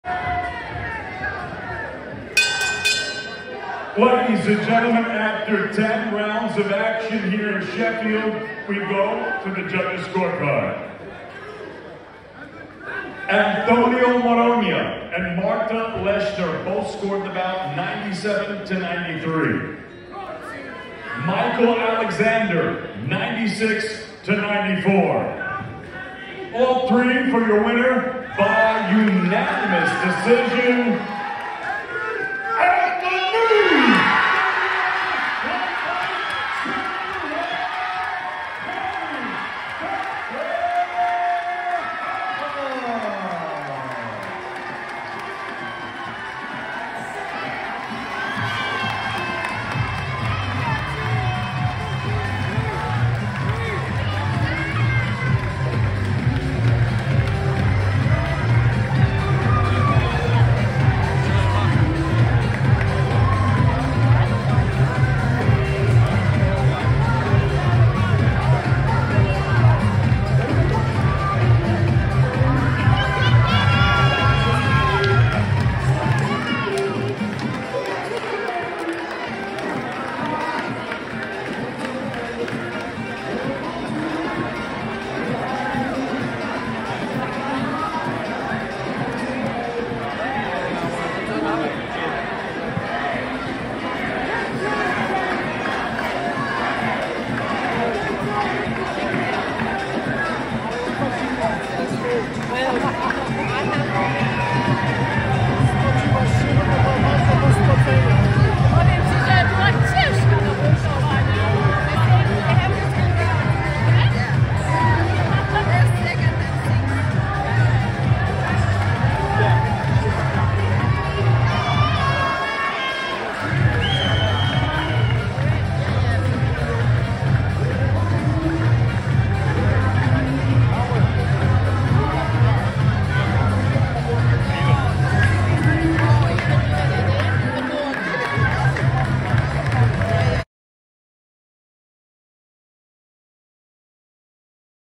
TERRI HARPER CELEBRATES AFTER BEATING RHIANNON DIXON
TERRI HARPER BECOMES WORLD CHAMPION AGAIN AFTER BEATING RHIANNON DIXON